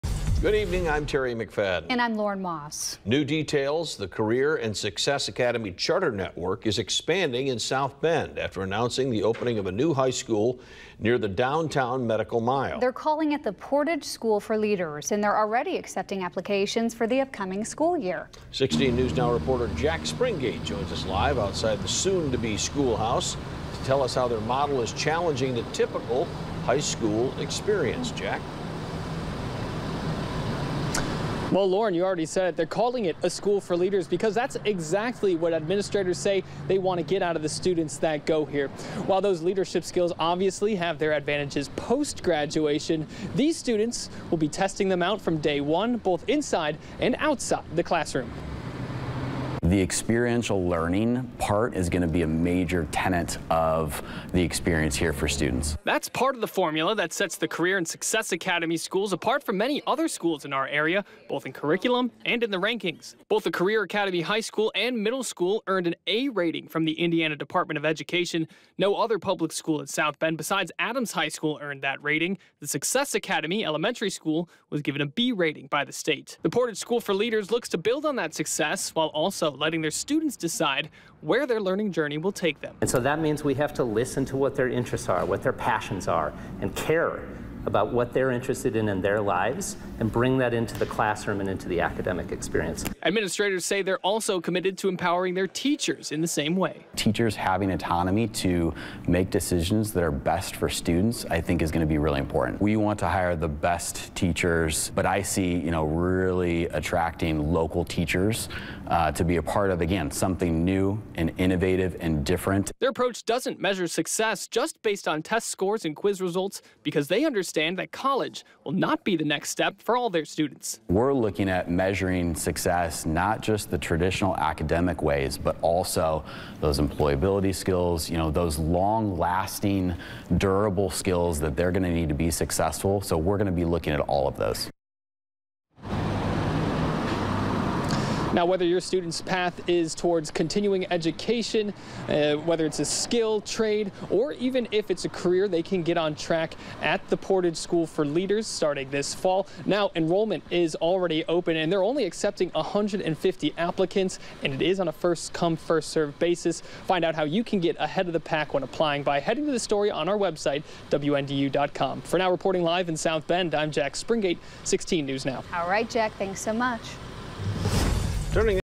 South Bend Career Academy announces new charter school
The Career and Success Academy charter network is expanding in South Bend after announcing the opening of a new high school near the downtown medical mile.